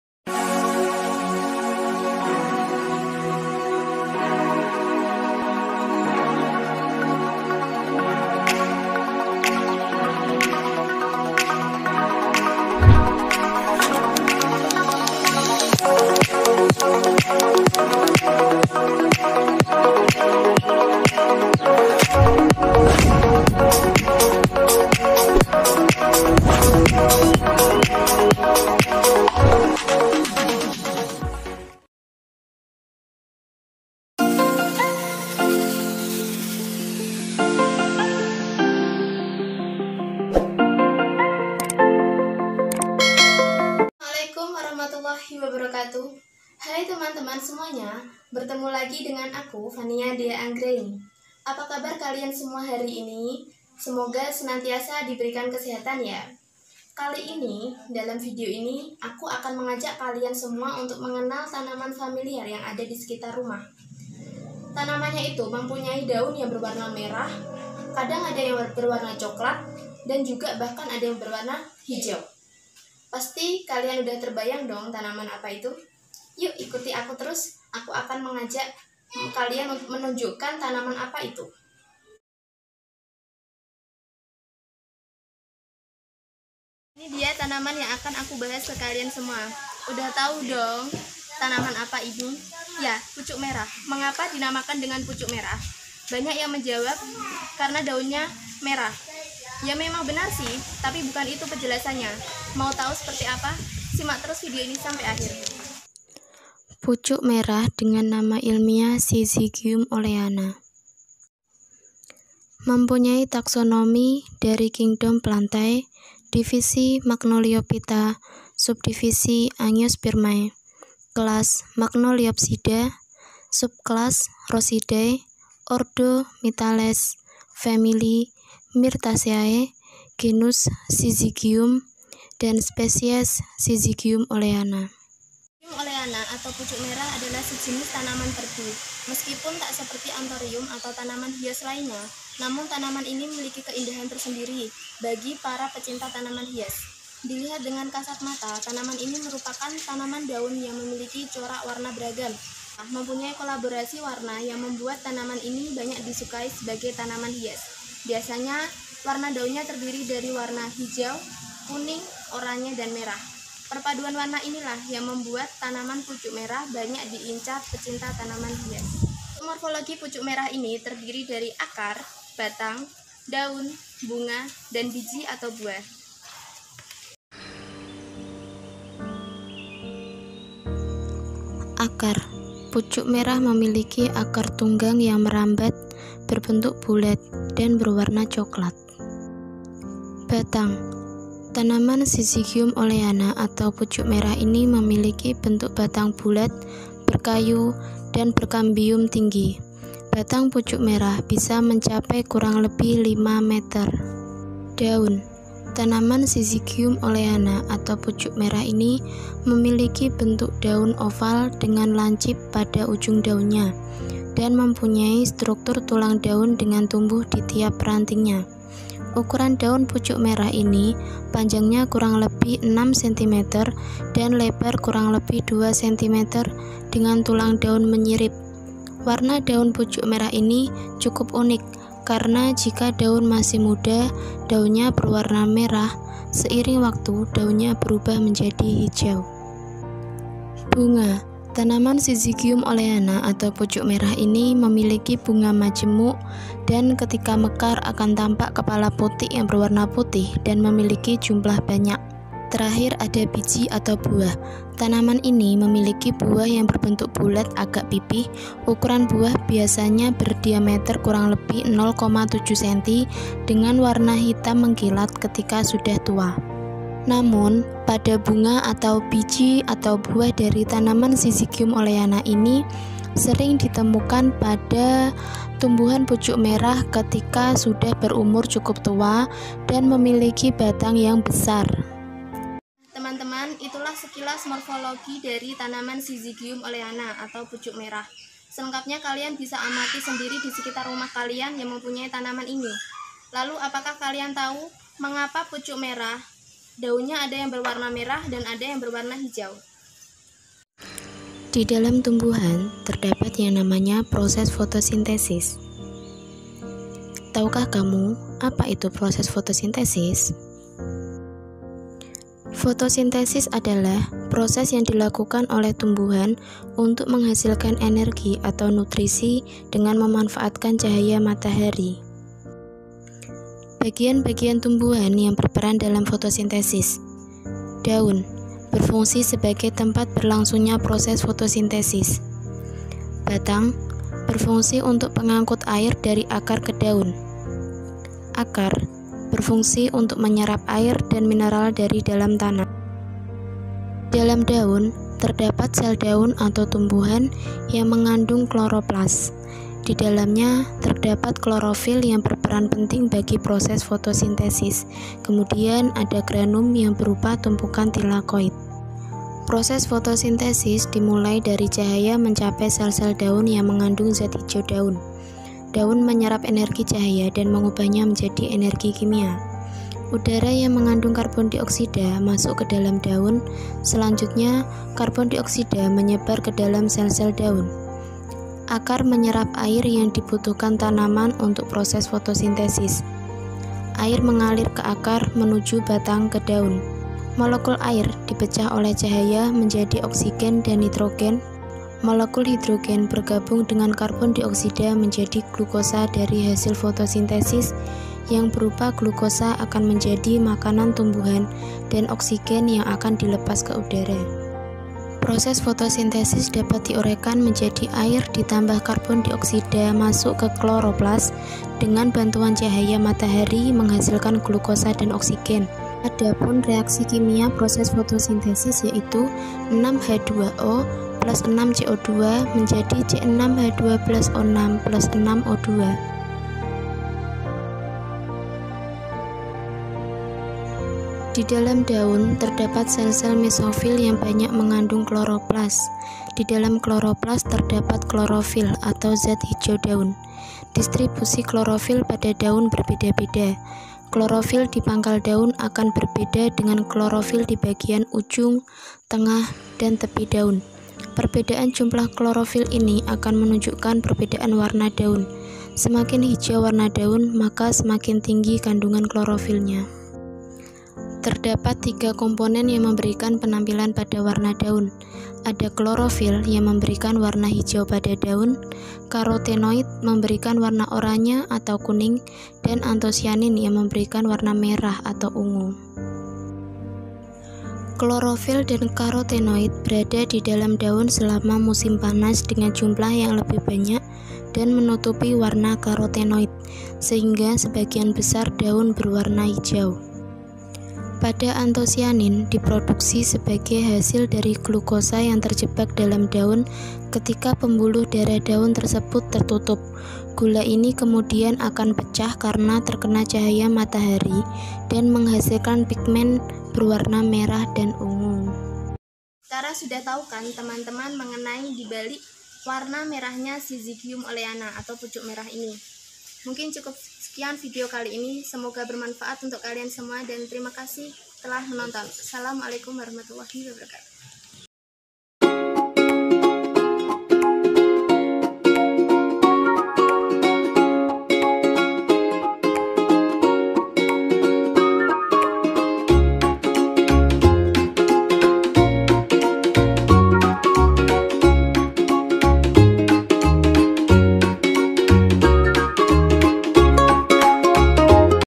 Dibalik Merahnya Syzygium oleana (Pucuk Merah) | KKN-IK-DR | TADRIS IPA | IAIN KUDUS | 81
Judul : Dibalik Merahnya Syzygium oleana
Vlogger : Vania Dhea Anggraeni
DPL : Ibu Hanik Malichatin, M.Pd

Haii haii kali ini aku akan ngenalin ke kalian semua tanaman hias yang ada disekitar rumahku. Daunnya mempunyai beragam warna ada merah, merah kecoklatan dan ada hijau. Apa itu??
Tanaman itu disebut dengan Syzygium oleana (pucuk merah). Sudah tidak asing lagi dong dengan tanaman itu??
Apakah kalian tau mengapa warna merah pada Syzygium oleana itu hanya di pucuk daunnya saja?
Itu disebabkan adanya proses "Fotosintesis"
Mau tau seperti apa prosesnya? Simak terus video ini sampai akhir.

Video ini dapat dijadikan sebagai sumber belajar tingkat SMP/MTs
3.2 kelas VII Mengklasifikasikan makhluk hidup dan benda berdasarkan karakteristik yang diamati.
3.5 kelas VII Menganalisis konsep energi, berbagai sumber energi dan perubahan bentuk energi dalam kehidupan sehari-hari termasuk fotosintesis
3.4 kelas VIII menganalisis keterkaitan struktur jaringan tumbuhan dan fungsinya serta teknologi yang terinsipirasi oleh struktur tumbuhan

IAIN KUDUS
TADRIS IPA
PUCUK MERAH
PERTANIAN KELUARGA